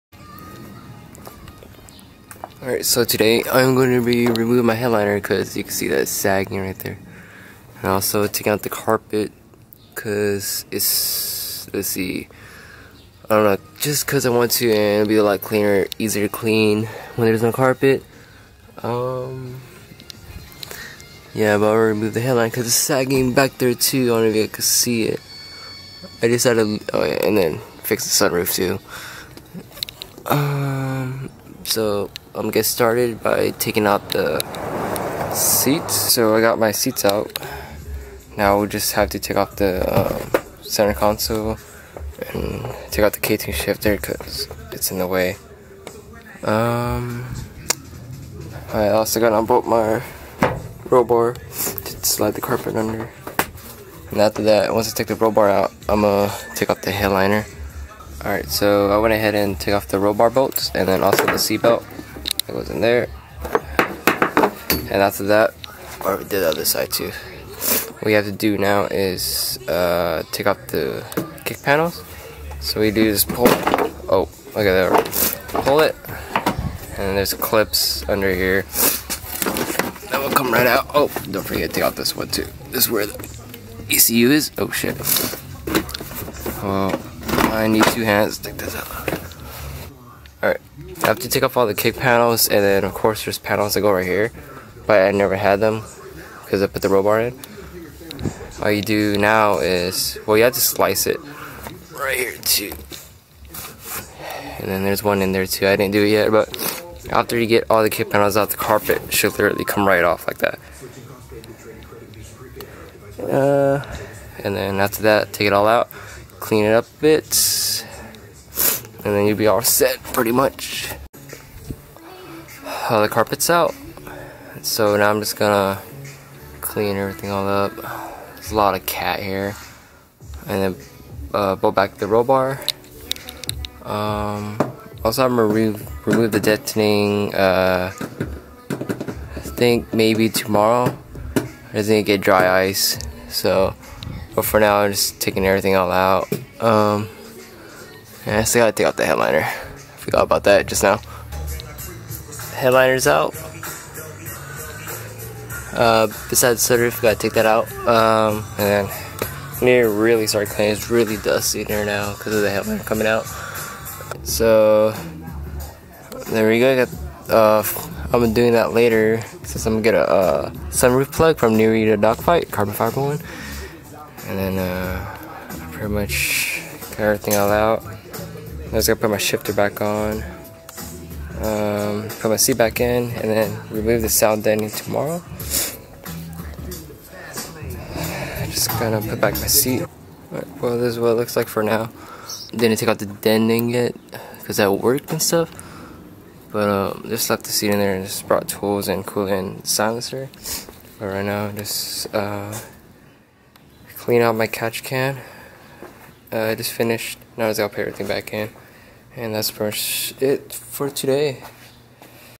Carpet Removal
Removing my dirty carpet and the saggy headliner.